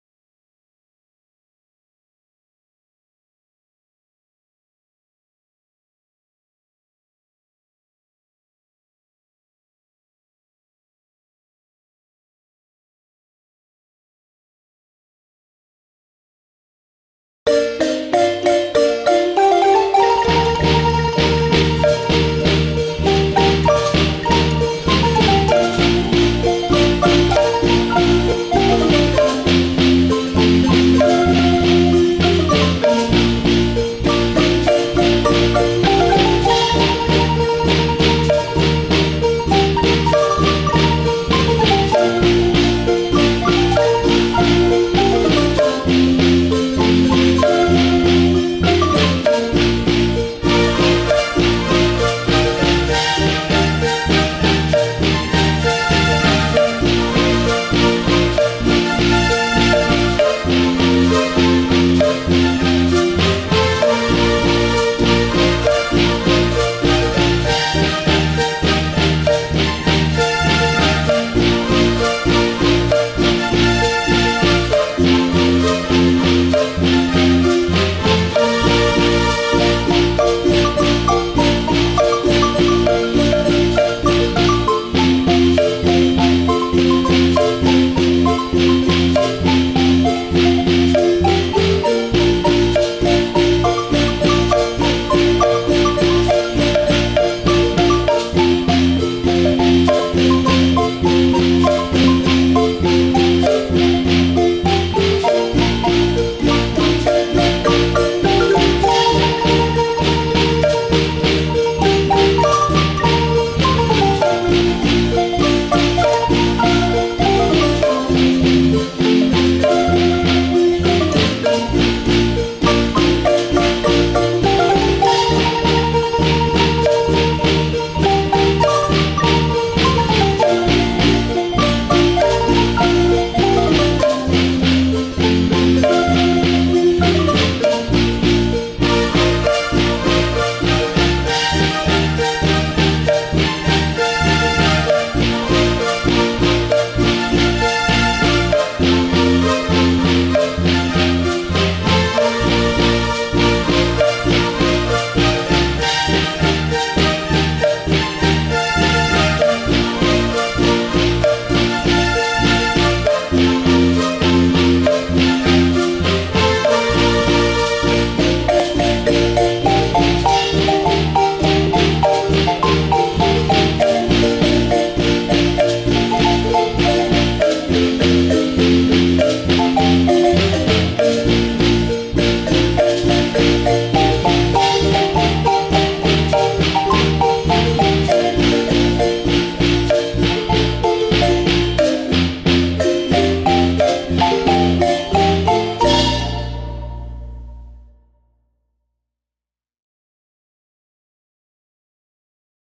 The Culture of the Maya in Guatemala-As seen through diverse colors
Guatemala is home to more than 23 spoken Maya Languages...Amongst the many of the world's greatest...Guatemala stands as one of the most diverse country in the world...Land of the marimba and the Maya